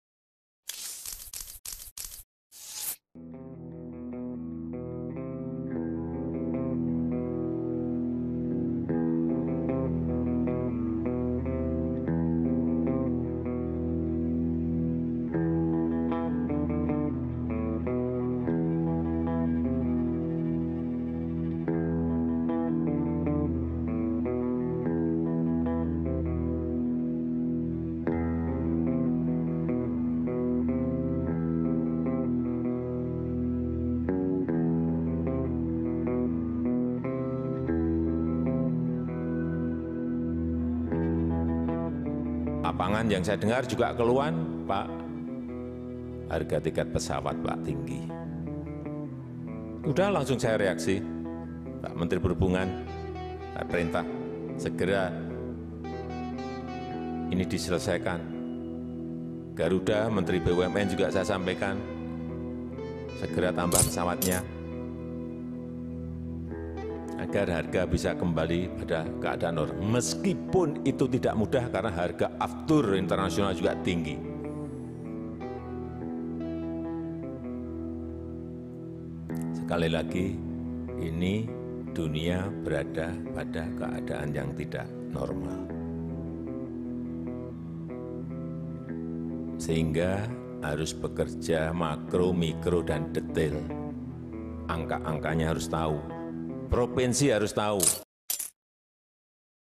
Jokowi Perintahkan Menhub Turunkan Harga Tiket Pesawat
Presiden Joko Widodo merespon keluhan masyarakat tentang harga tiket pesawat yang mahal di Indonesia. Dalam rapat di Istana Negara, Jokowi memberikan perintah langsung ke Menteri Perhubungan Budi Karya Sumadi dan Menteri BUMN Erick Thohir dalam rapat koordinasi nasional pengendalian inflasi di Istana Negara.

Kenaikan harga tiket pesawat selama ini disebut karena tingginya harga avtur internasional. Sehinga maskapai menaikkan harga tiket pesawat untuk tetap beroperasi. Namun masyarakat merasa kenaikan harga tiket pesawat terlalu tinggi saat ini.

Merespon perintah Jokowi, Budi Karya Sumadi berencana mencari solusi dengan menggandeng pemerintah daerah untuk ikut memberikan bantuan salah satunya dengan mensubsidi.